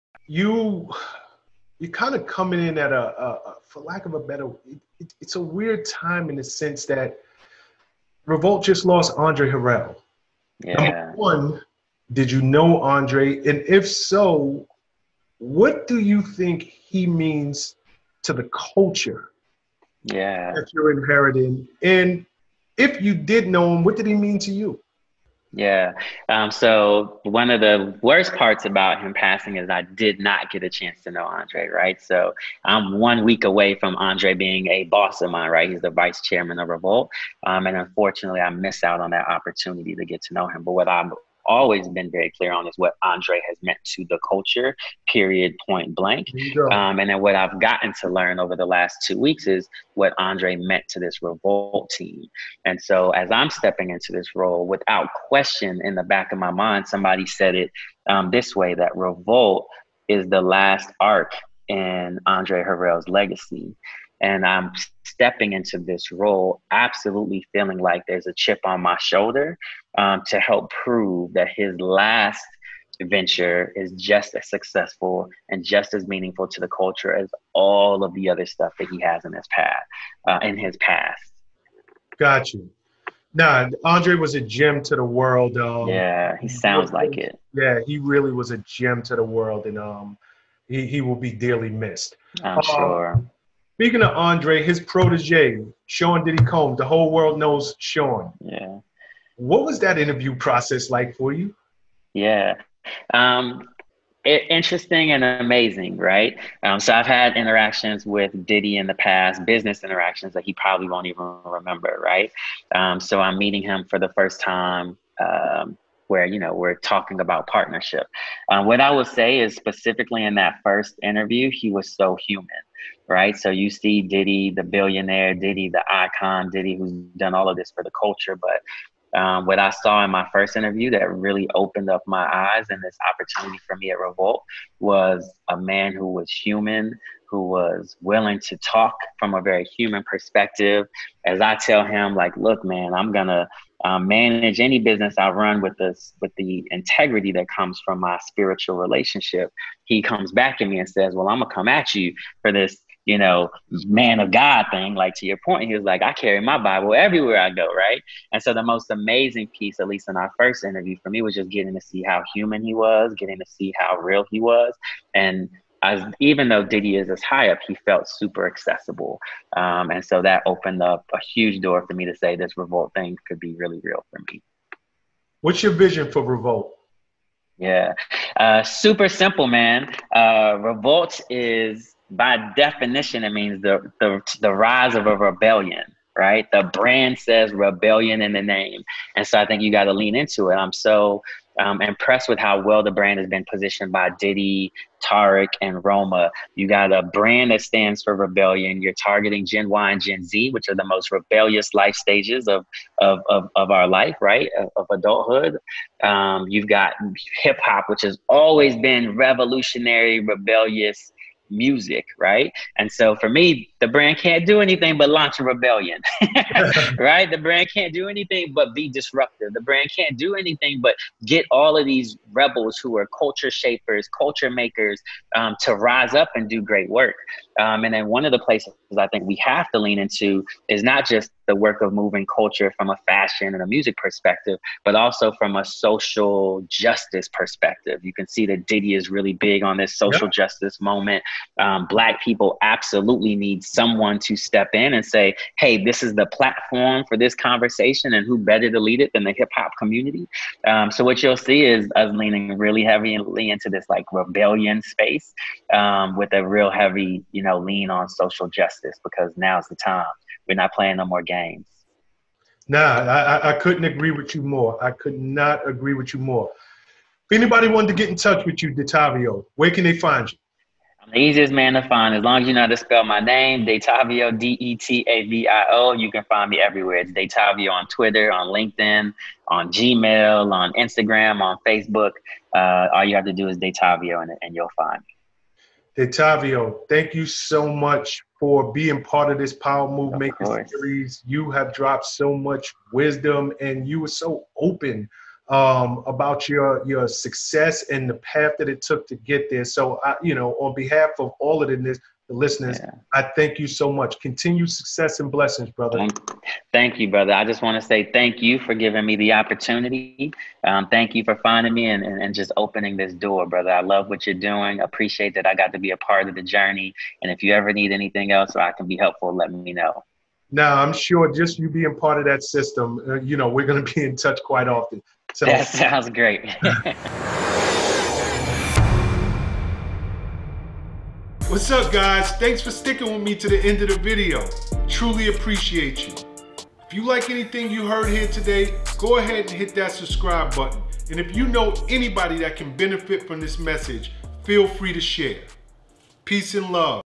THE RISE OF A REBELLION - DETAVIO SAMUELS
Detavio Samuels recently took the position as COO of Revolt Media & TV. He views REVOLT as the last arch in Andre Harrell's legacy. In this segment, Detavio talks about interviewing with Diddy for the job.

Detavio also discusses his vision for the media group. In his eyes, Revolt by definition is disruption. It's a priority to move Black culture forward from a social justice perspective. REVOLT will be the platform Black people will leverage to have important conversations about their community.

Listen in for a powerful discussion about culture and business.

Twitter/Linkedin/Insta/FB Handle - Detavio